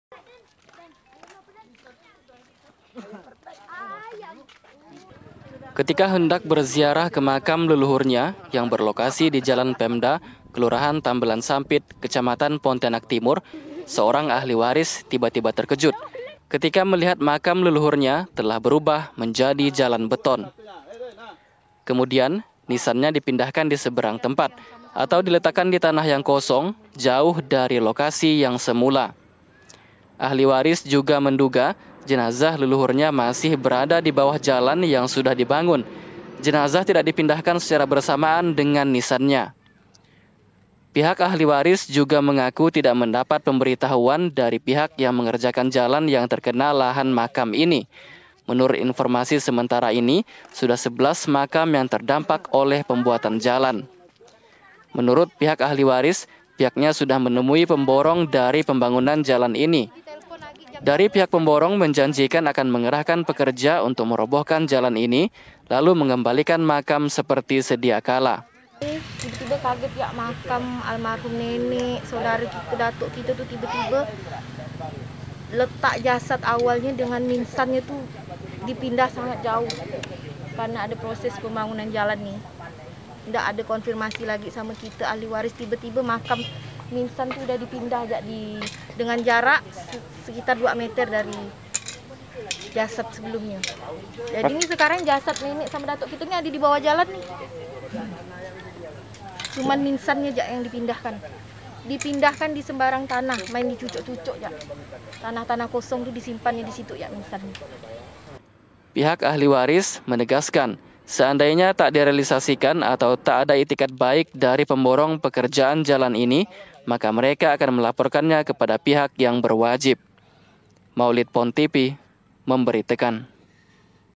AHLI WARIS TERKEJUT LIHAT MAKAM LELUHUR JADI JALAN
Seorang ahli waris pemilik lahan wakaf makam terkejut, saat berziarah ke makam leluhurnya di Jalan Pemda, Kelurahan Tambelan Sampit, Pontianak Timur. Pasalnya makam tersebut kini telah berubah menjadi jalan beton. Nisan makam juga telah dipindahkan jauh dari lokasi awal, dan kuat diduga jenazahnya masih berada di bawah jalan beton itu. Ahli waris mengaku pembangunan jalan di lokasi makam tidak ada pemberitahuan kepada ahli waris